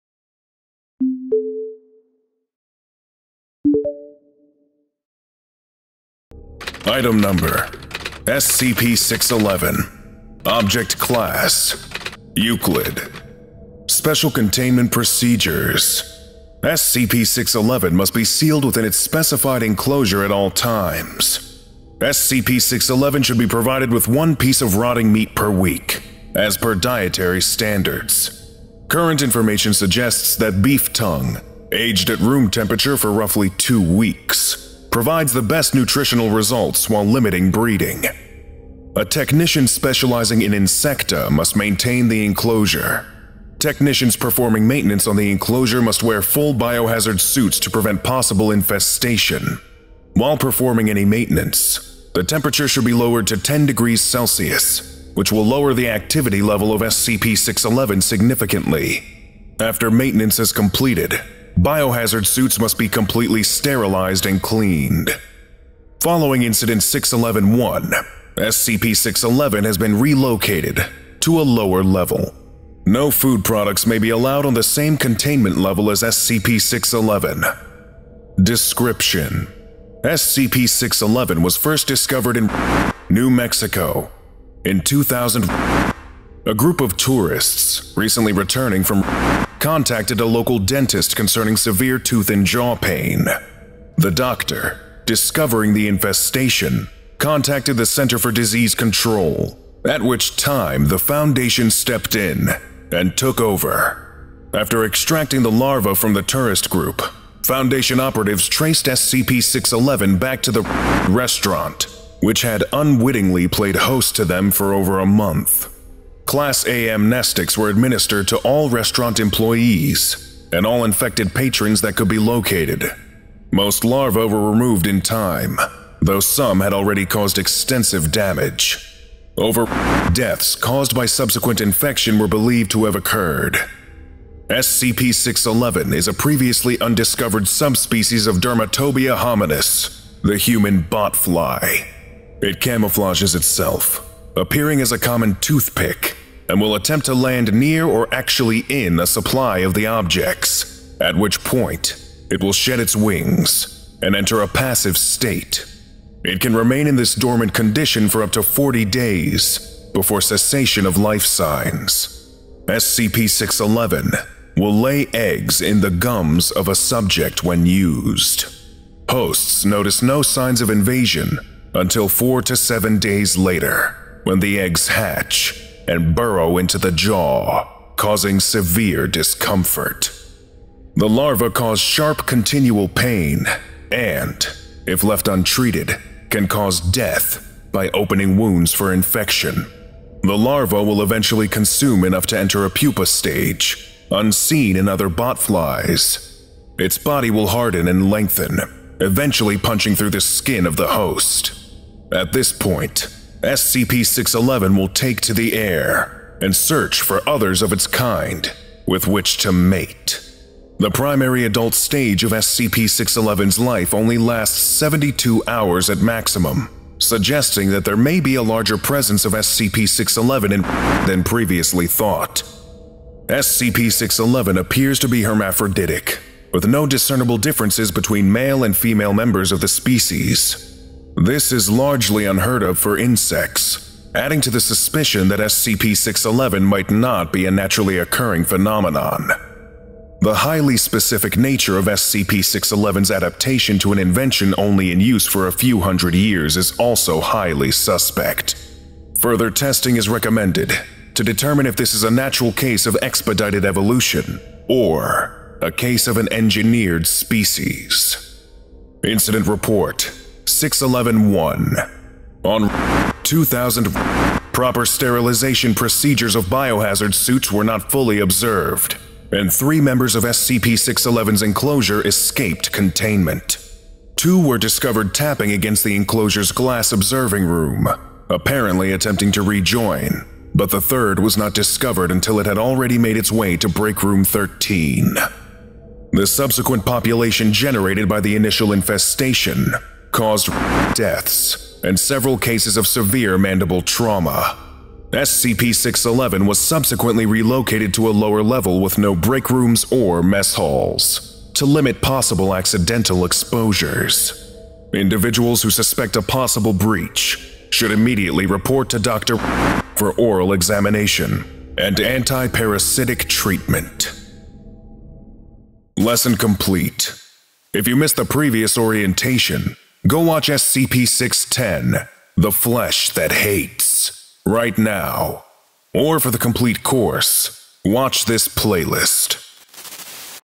SCP-611 | Parasitic Toothpick (SCP Orientation)
Today we will be studying Item number SCP-611: Parasitic Toothpick, Object class: Euclid.

SCP-611 is a previously undiscovered subspecies of the human botfly. It camouflages itself, appearing as a common toothpick, and will attempt to land near or actually in a supply of the objects, at which point it will shed its wings and enter a passive state. SCP-611 will lay eggs in the gums of a subject when used. Hosts notice no signs of invasion until four to seven days later when the eggs hatch and burrow into the jaw, causing severe discomfort.

and released under Creative Commons Sharealike 3.0. Contributor: TroyL

Attribution:  Mstyslav Chernov